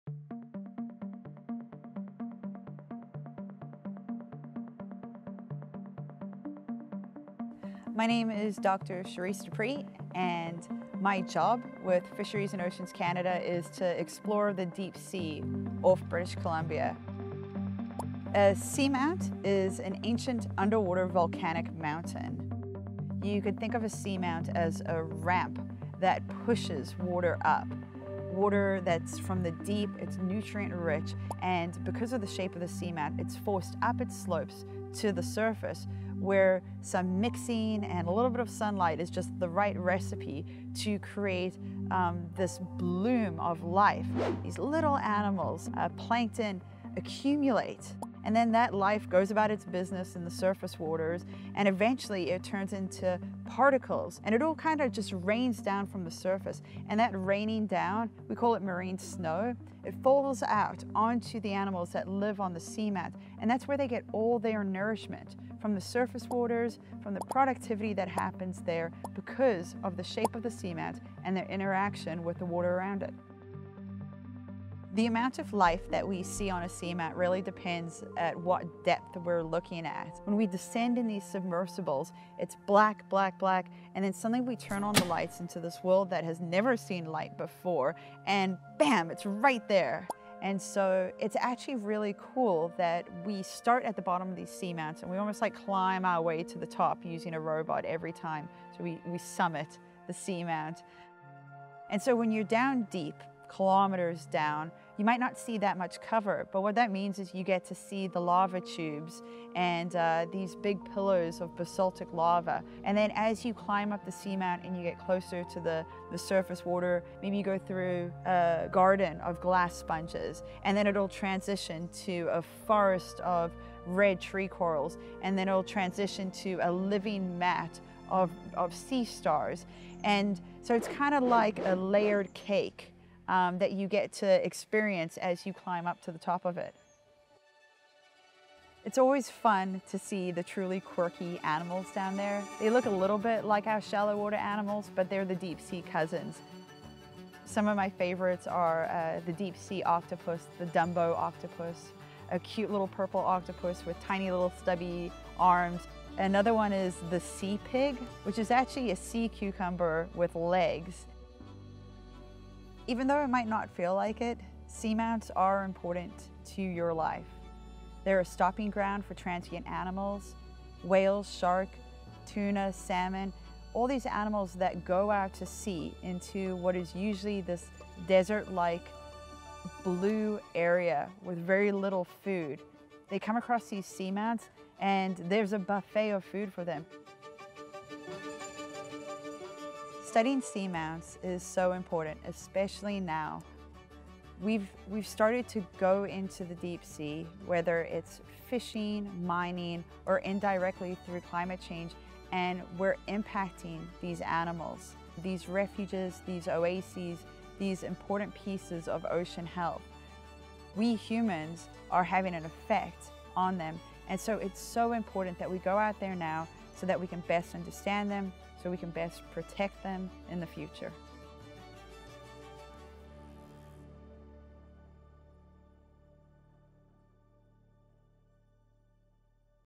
Summiting Seamounts
Seamounts are ancient underwater volcanic mountains created by the shifting of plates around the globe. These impressive underwater ranges rival the Rocky Mountains for their grandeur, except for one thing: they’re completely hidden from sight—until now. Scientists in Canada have discovered 62 seamounts off the coast of British Columbia, most of which were discovered in just the last three years. And it turns out they host some of the most bizarre creatures on the planet.

Cherisse Du Preez, from Fisheries and Oceans Canada, takes us on a deep dive of these incredible oases of life in the vast ocean blue.

If you enjoyed this video, you’ll find lots of related stories on our You can sign up for our free weekly newsletter

Producer/Editor/Camera: Katrina Pyne
Expedition footage provided by Fisheries and Oceans Canada, Northeast Pacific Seamounts Expedition Partners, Ocean Exploration Trust, and Shelton Dupreez
Additional footage provided by Ocean Networks Canada